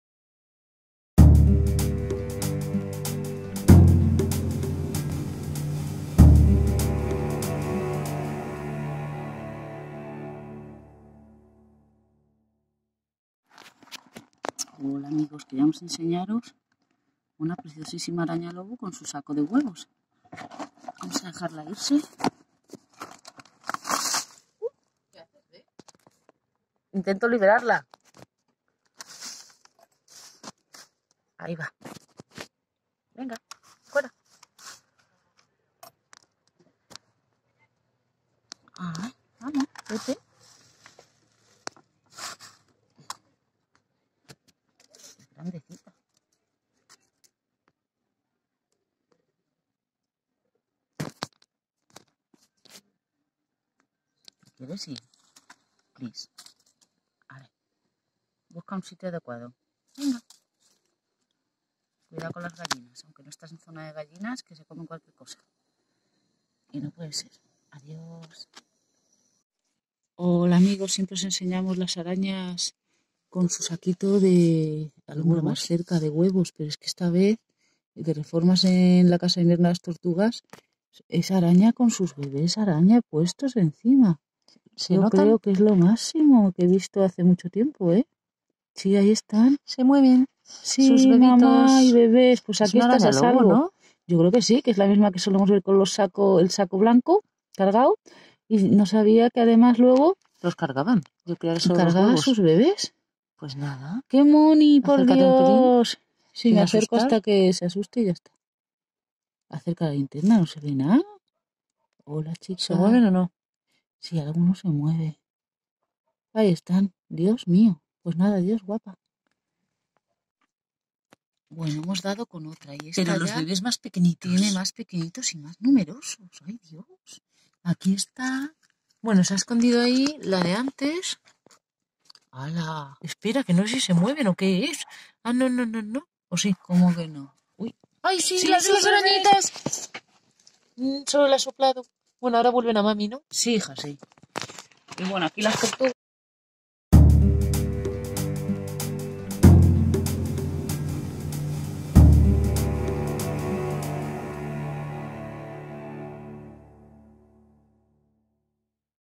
Araña lobo (Lycosa tarantula) y sus crías - saco sedoso y crías en abdomen
Es una tarántula europea y es la mayor araña del Viejo Continente: las hembras miden de 27 a 30 mm y los machos de 19 a 25 mm.

La hembra de la araña lobo vive en su gruta, desde la que caza agazapada en la entrada. La cueva es en realidad una galería vertical que puede llegar a una profundidad de 30 cm, situada en terrenos pedregosos y soleados. Su entrada suele estar forrada de hierbas y palitos unidos con seda, formando un pequeño embudo.

Las arañas lobo son nocturnas, aunque la hembra puede ser vista a la entrada de su cueva durante todo el día durante el buen tiempo. Se aparean en primavera, y el resultado es un centenar de huevos que la hembra envuelve en seda.

Cuando los huevos eclosionan, llevan a las crías sobre el abdomen hasta que realizan su primera muda.

Los machos pueden llegar a vivir hasta dos años, muriendo poco después de alcanzar la madurez sexual. Las hembras, sin embargo, alcanzan los cuatro años de vida o más.